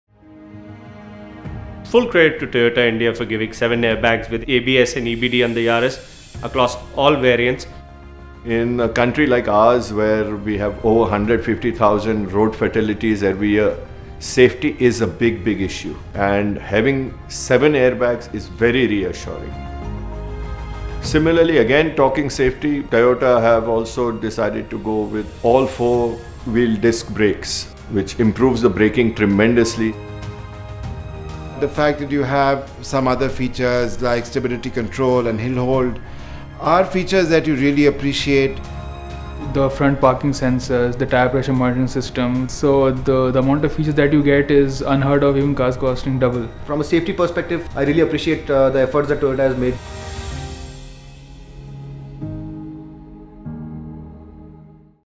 Toyota Yaris – The new benchmark for Safety
With standardised 7 SRS airbags, ABS + EBD, all 4 Disc Brakes and more, the Toyota Yaris is easily a new benchmark for safety!